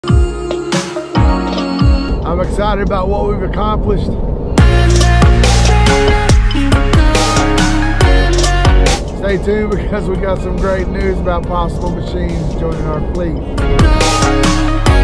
December 2, 2021
In this Video, Travis with Trail Kings breaks down why Trail Kings stopped posting videos. Stick around to the end to find out if there will be anymore. A lot has happened.

📒 Our Tools and Accessories List for any SXS owner 📒